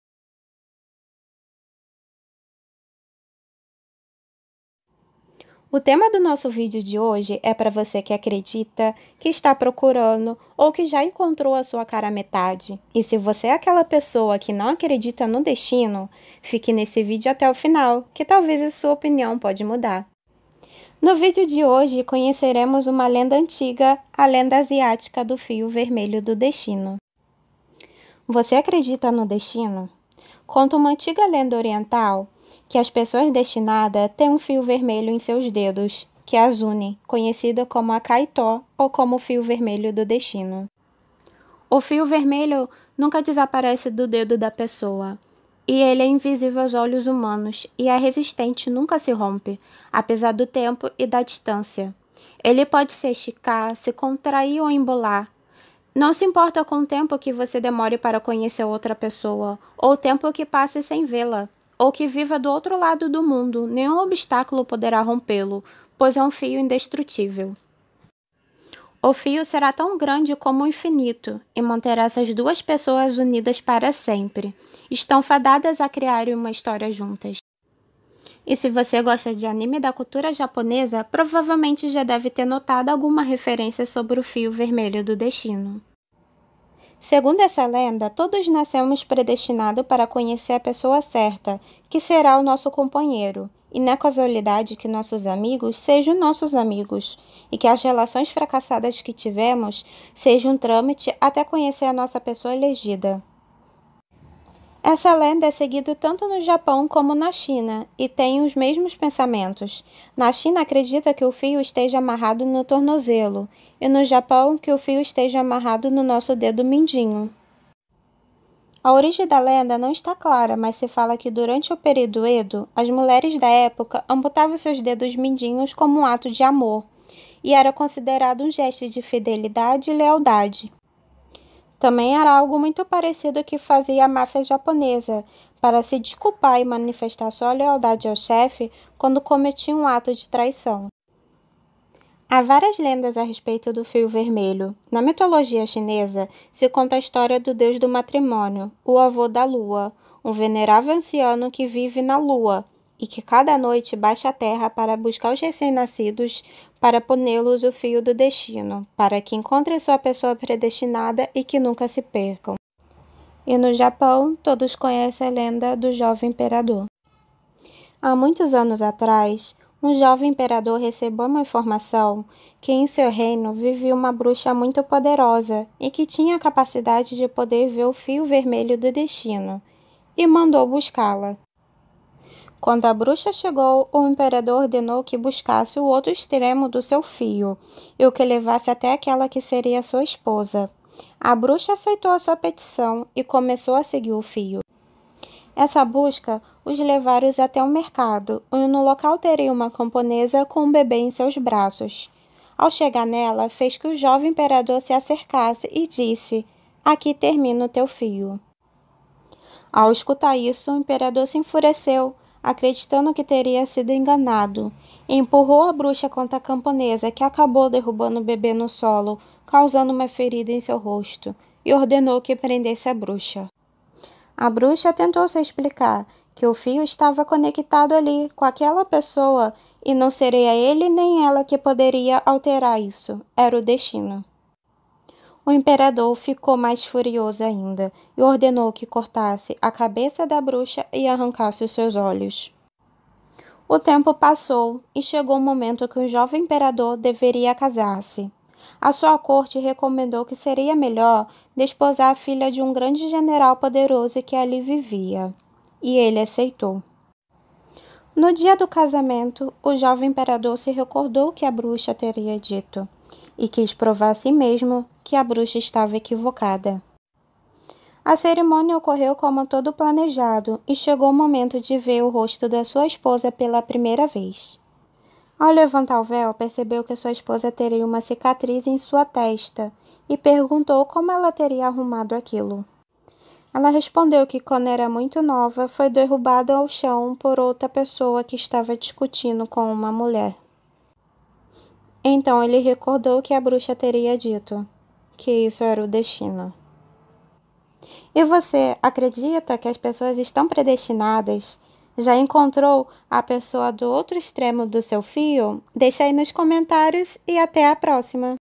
O FIO VERMELHO DO DESTINO
🍿 SINOPSE 🍿📽
O TEMA DO NOSSO VIDEO DE HOJE É PARA VOCE QUE ACREDITA, ESTA PROCURANDO OU JA ENCONTROU SUA ALMA GÊMEA.
E SE VOCE NAO ENCONTROU OU NÃO ACREDITA FIQUE ATE O FINAL QUE SUA OPNIAO PODE MUDAR

Classificação: ✅-   recomendado para menores de 18 anos.

Gênero: Fantasia animada